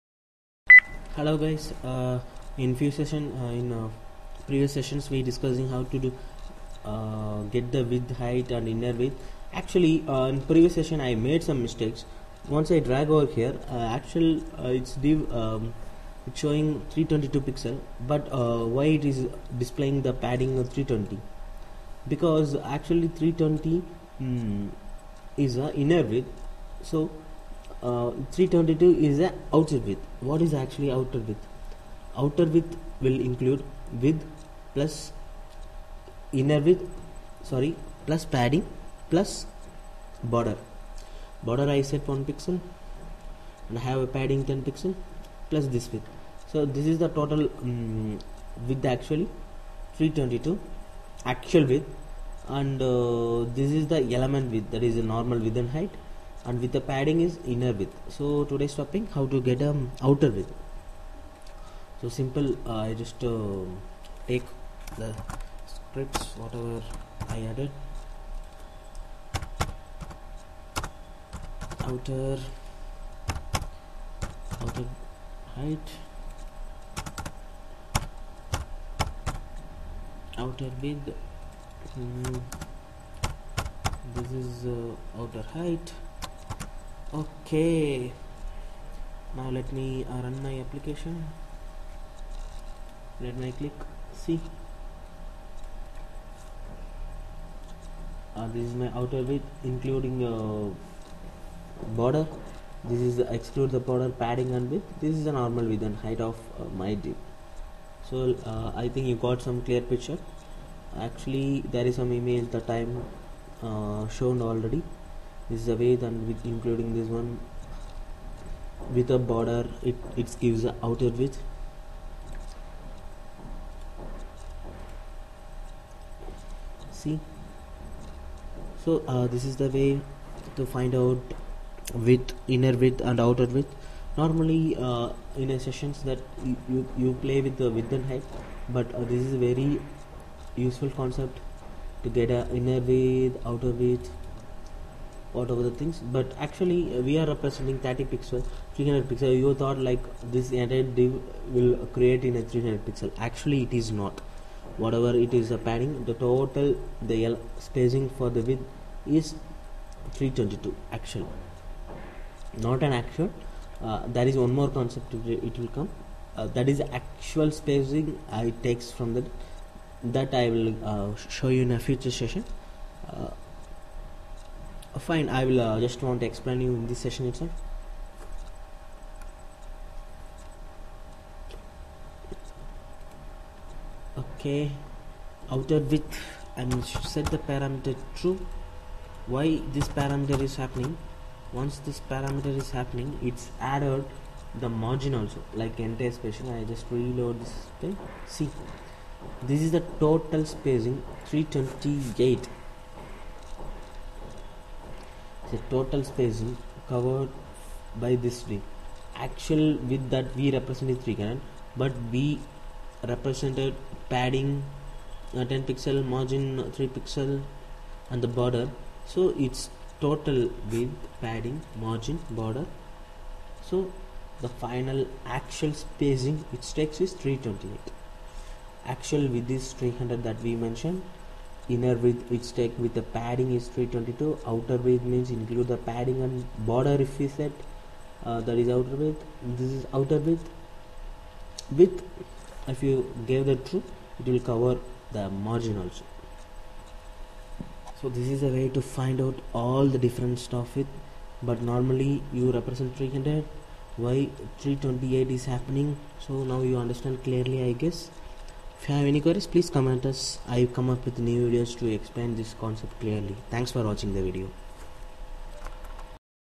What is difference between width and outerWidth, height and outerHeight in jQuery
What is difference between width, innerWidth and outerWidth, height, innerHeight and outerHeight in jQuery.

The outerWidth() method returns the width of an element (includes padding and border).

The outerHeight() method returns the height of an element (includes padding and border).